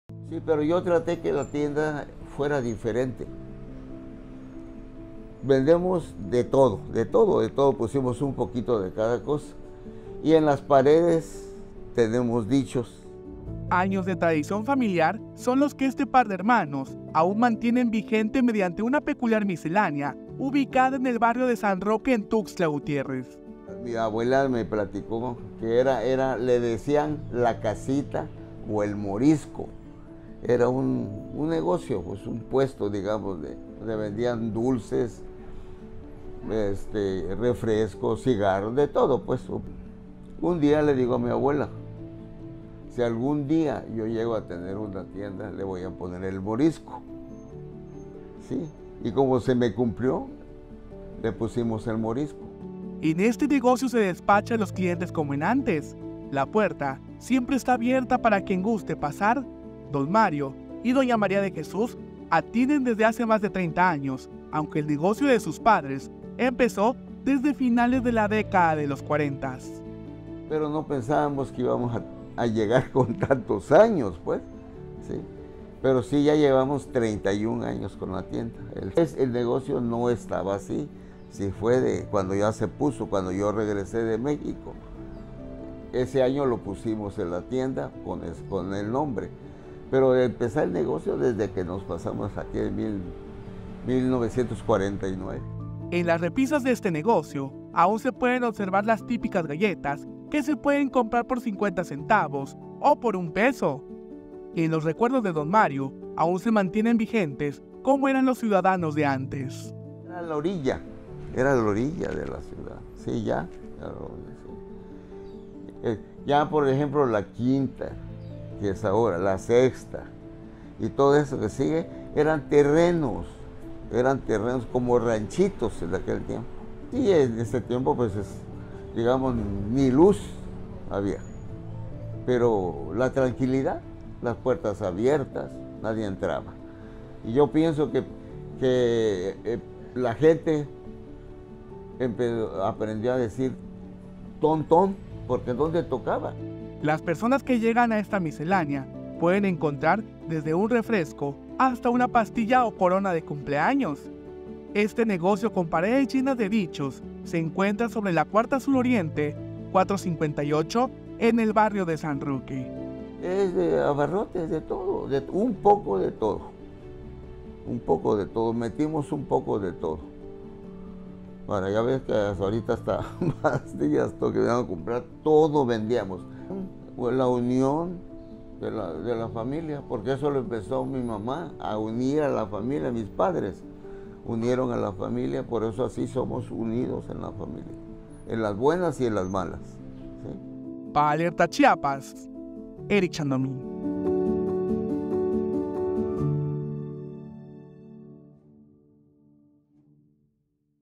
¿Cuántos dichos dicen que has dicho?
¿Conoces la miscelánea tuxtleca que sus paredes están llenas de dichos?